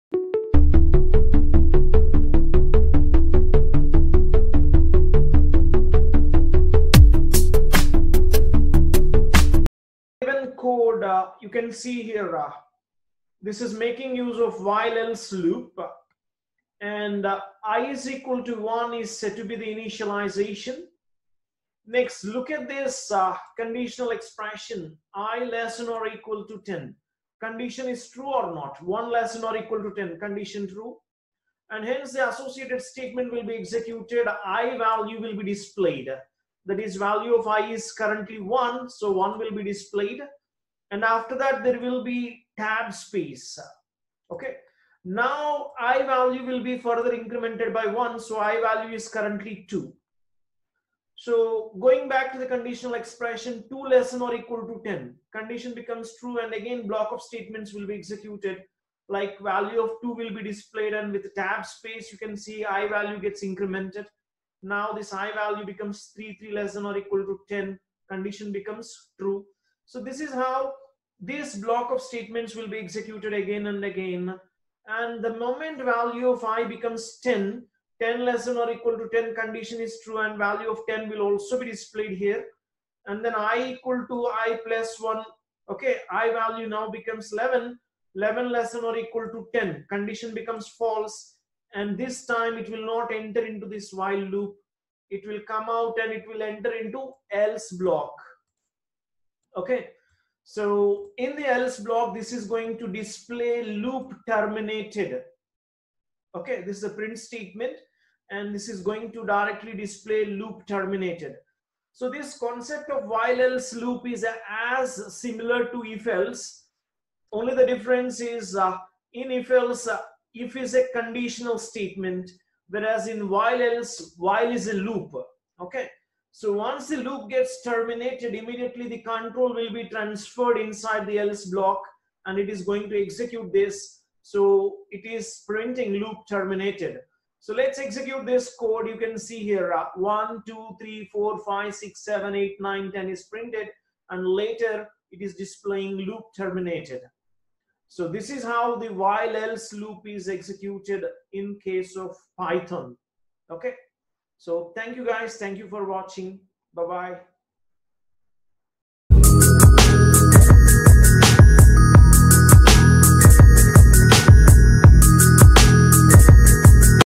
While Loop with Else Clause in PYTHON Programming
In this video we will see an example of While Loop with Else Clause. The Python Programming supports to have an else statement associated with a loop statement. If the else statement is used with a while loop, the else statement is executed when the condition becomes false.

Want to join Python Programming Online Course then register in our course